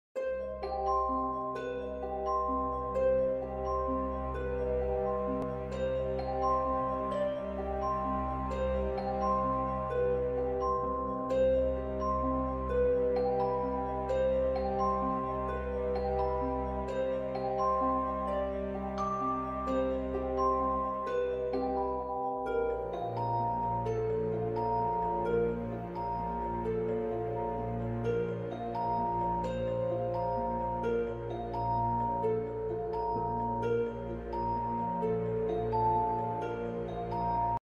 Open Recruitment UKPM Genta Andalas 2016
Open Recruitment UKPM Genta Andalas 2016, dibuka mulai 4-17 April 2016. More Info twitter: @Genta_Andalas